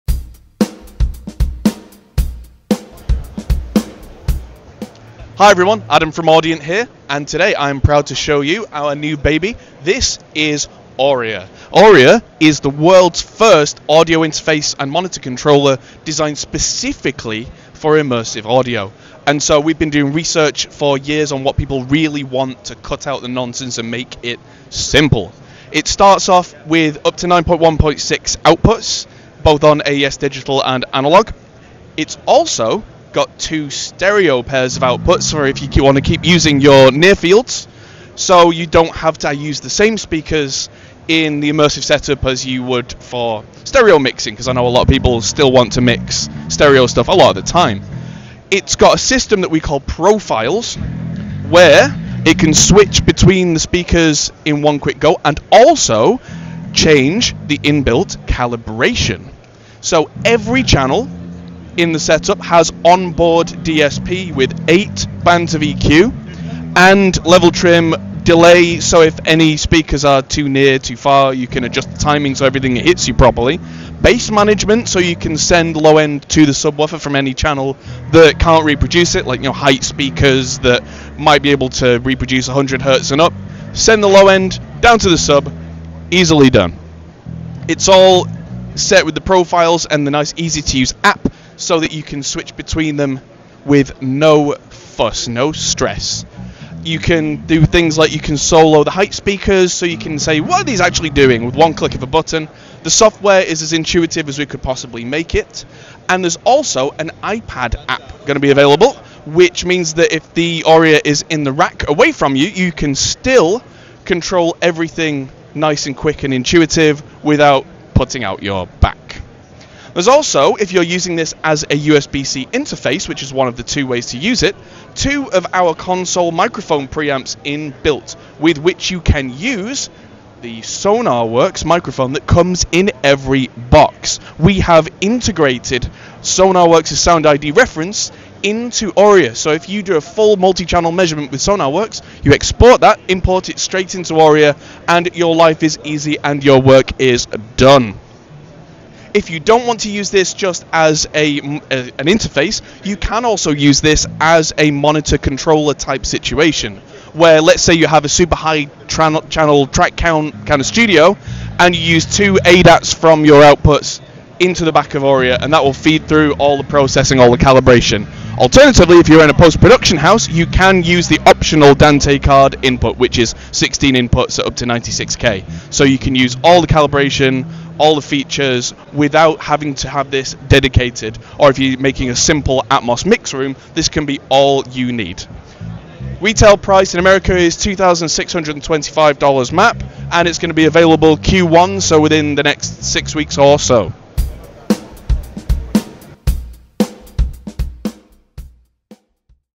Dale Pro Audio - Audient ORIA Immersive Monitor Controller at Winter NAMM 2024
Adam Steel from @Audientworld introduces the new ORIA immersive monitor controller/interface at the 2024 Winter NAMM show.